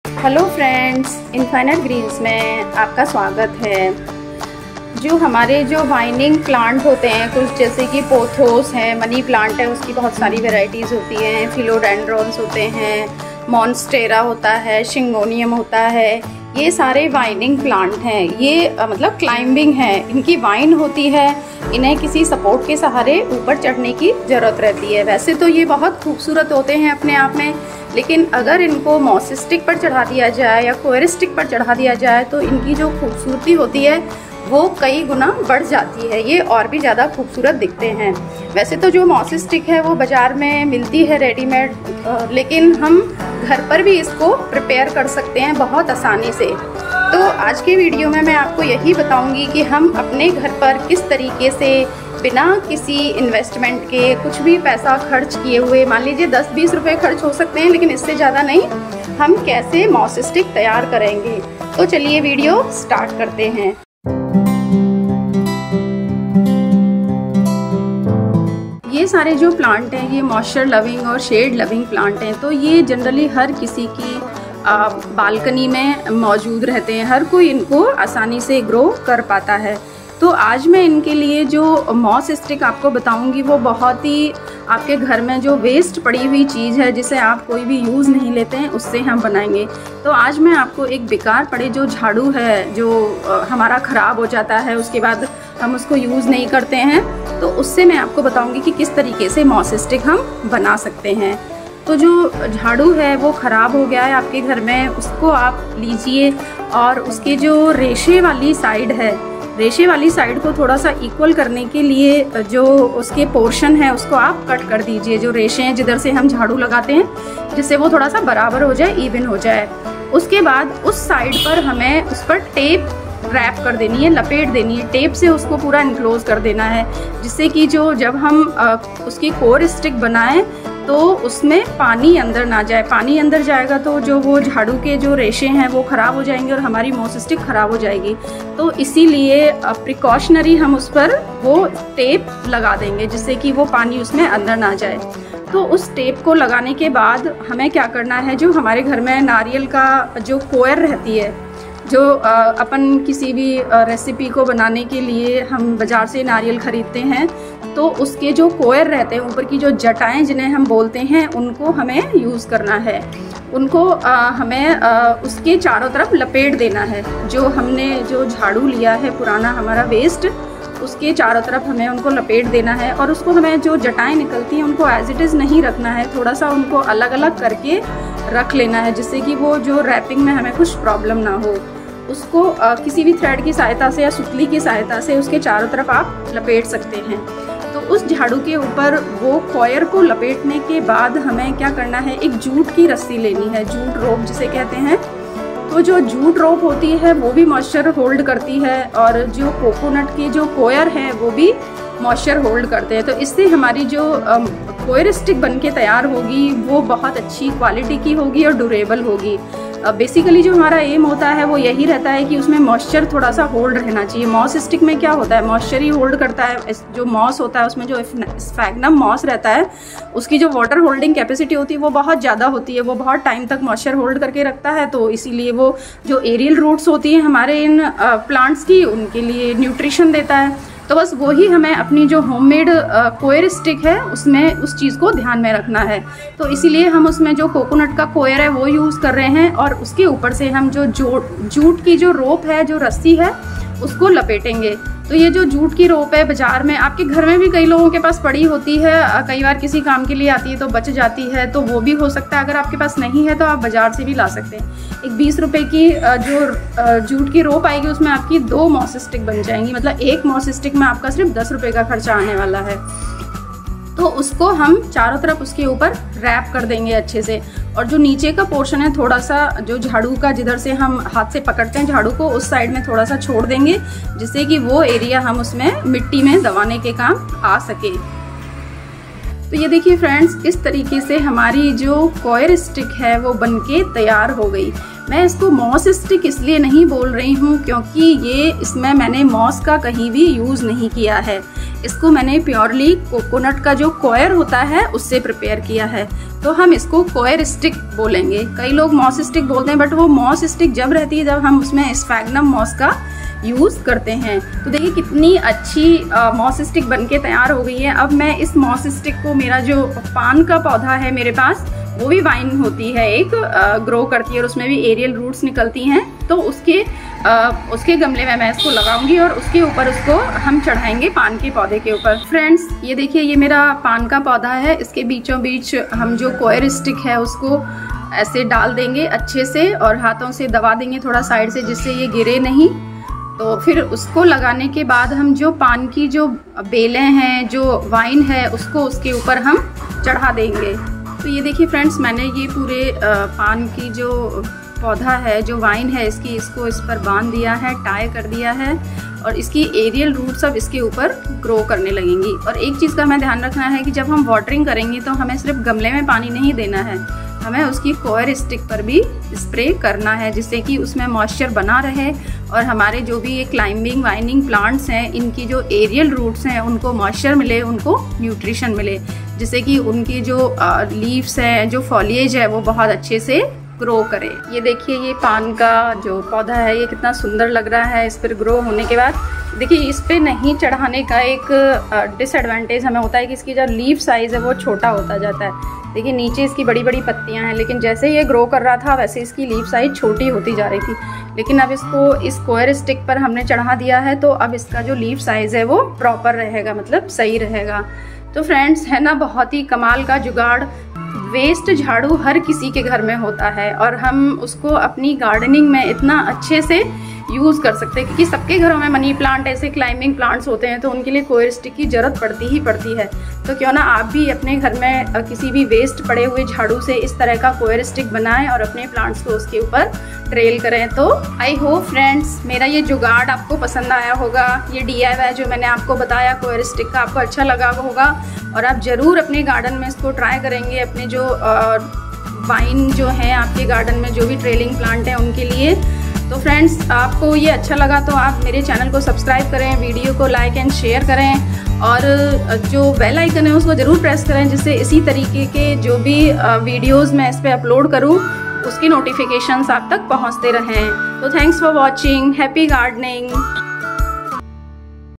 Coir stick/Moss stick/DIY/Best use of old Broom,पुराने झाड़ू का सबसे अच्छा उपयोग ,Money plant
In this video I will show how to use old broom to make a coir stick for plants.